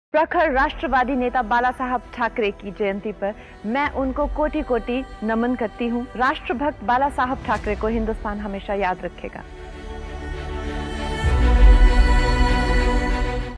Kangana Ranaut remembers Bala Saheb Thackeray on his birth anniversary
KanganaRanaut shows she's the true Queen as she remembers Bala Saheb Thackeray on his birth anniversary ahead of the Box Office clash between Manikarnika & Thackeray.